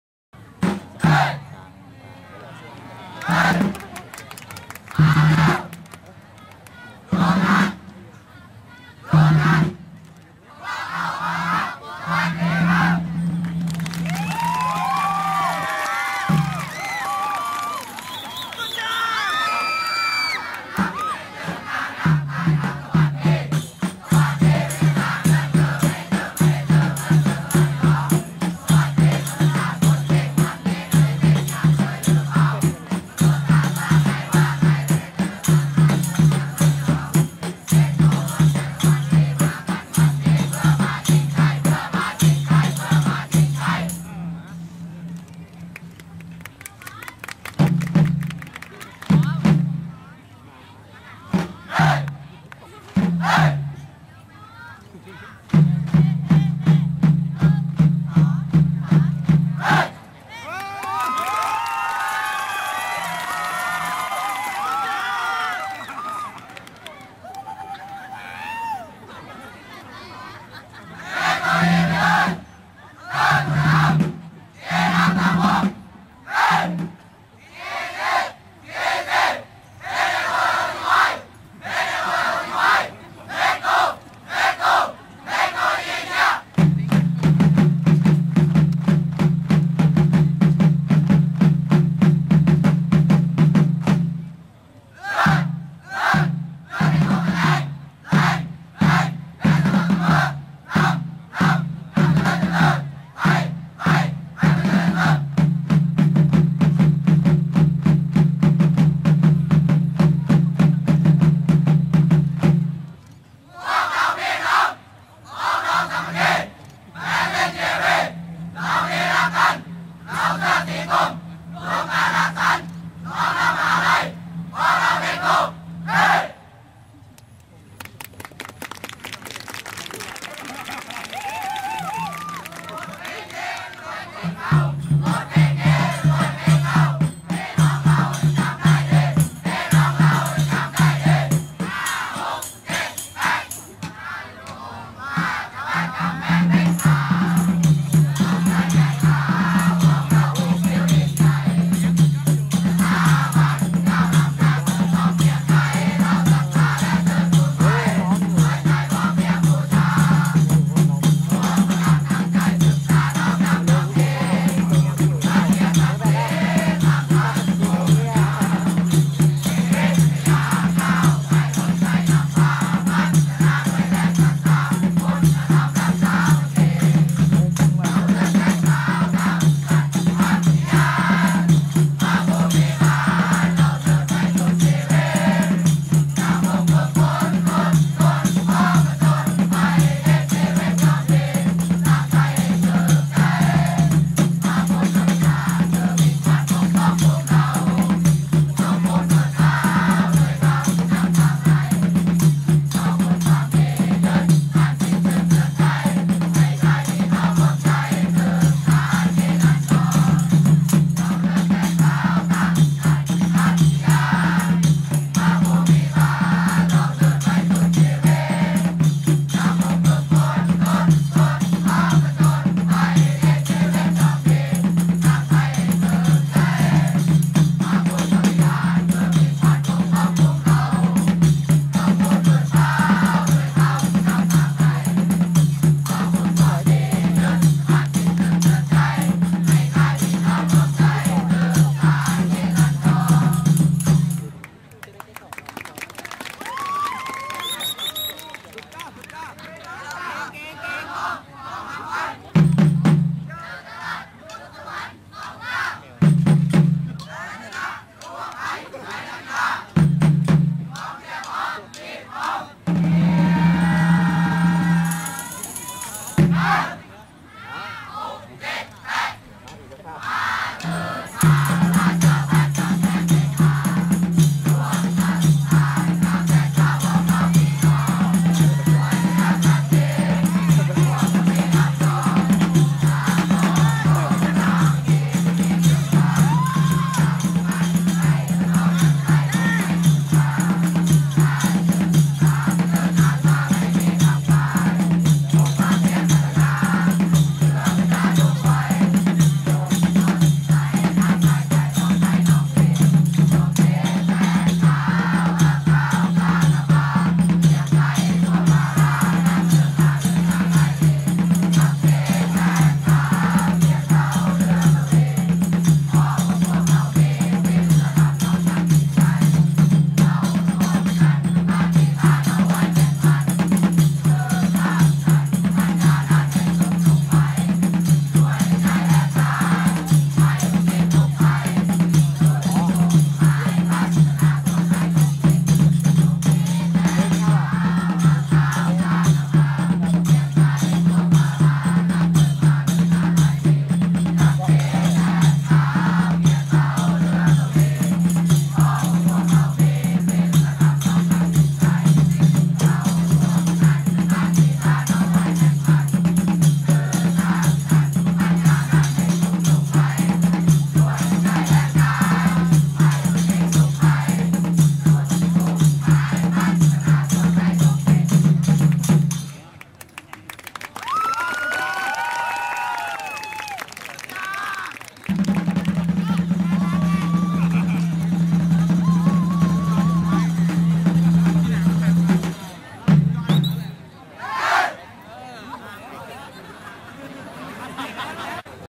คณะเทคโนโลยี
คณะเทคโนโลยี ในการแข่งขันกีฬาภายใน (ปลาบึกเกมส์) ครั้งที่ 11 มหาวิทยาลัยราชภัฏกำแพงเพชร วันที่ 31 สิงหาคม 2554